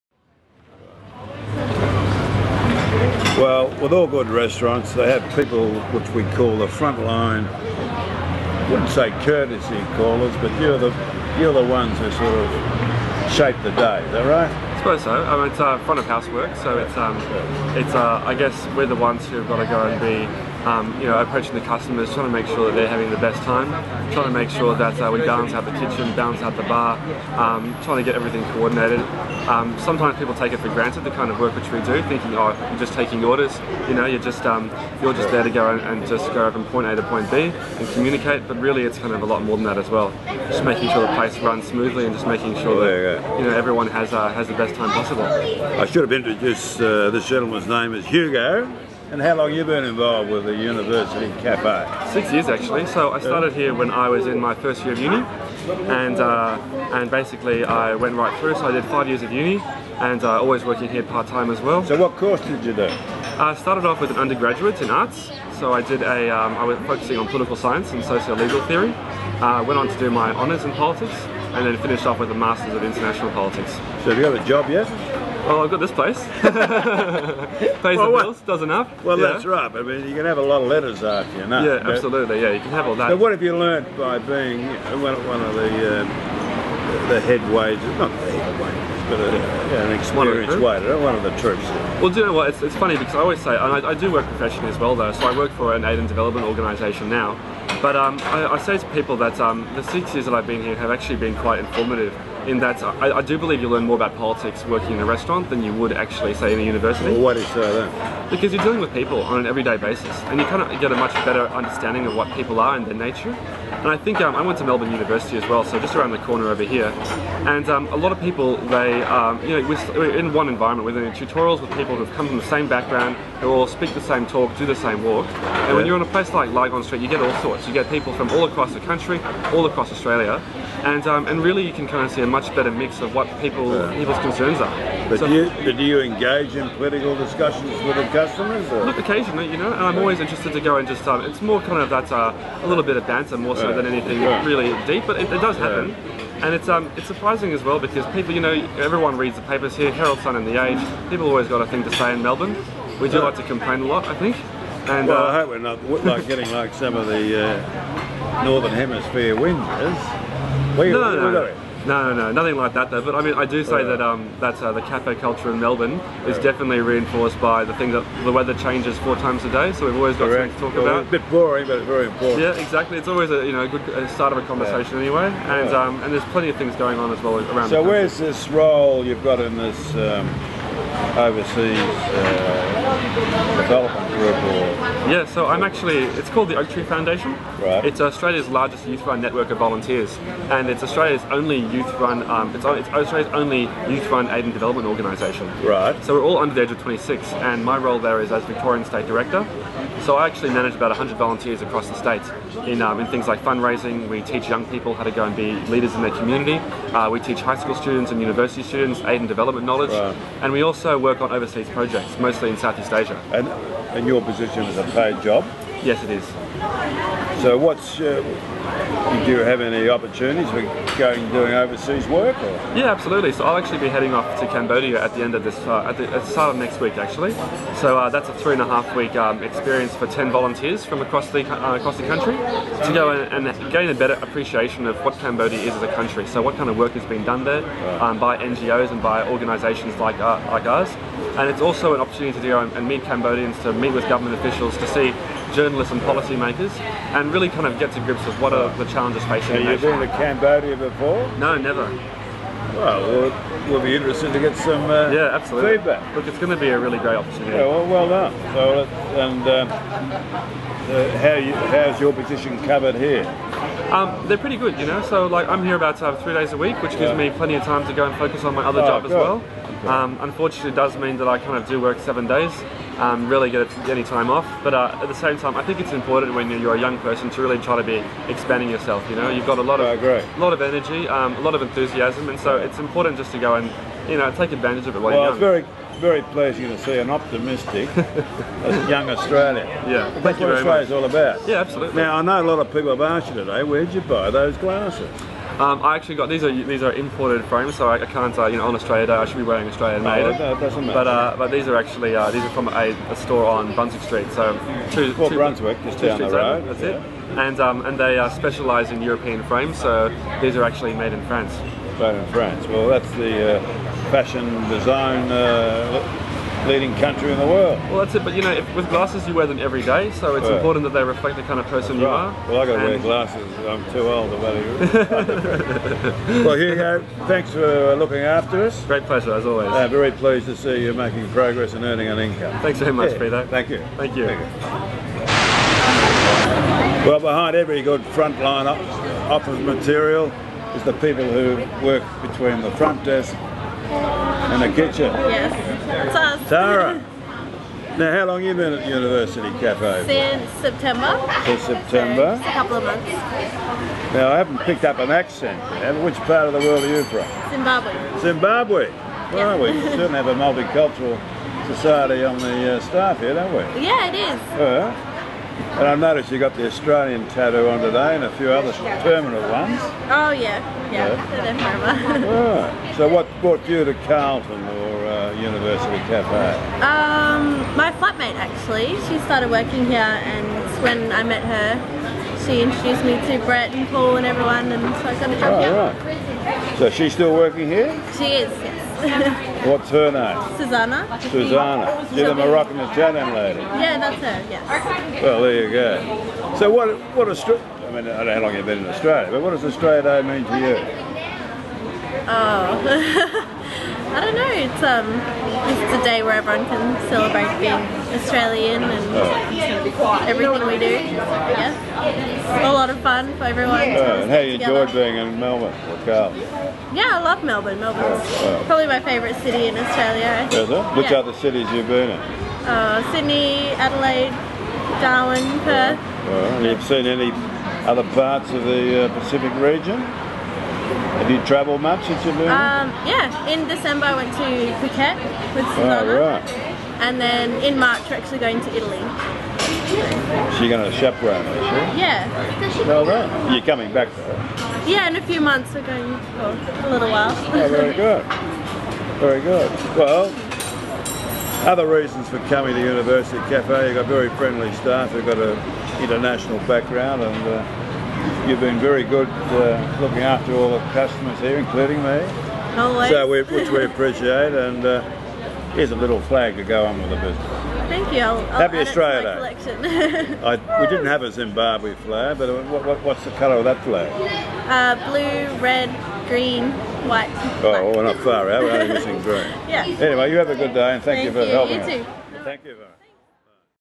More Aussie Day Interviews At University Café In Lygon Street Carlton
When the John Elliott film crew were at University Café on Australia Day where Broo premium lager was the feature beer, we interviewed two popular University Café  staff members, Tara and Hugo (whose kinky red glasses attracted a lot of attention from customers).

It was a fun day and you will enjoy these filmed interviews in the video below.

Special thanks to Broo and the University Café management and staff for supporting this special Aussie Day promotion.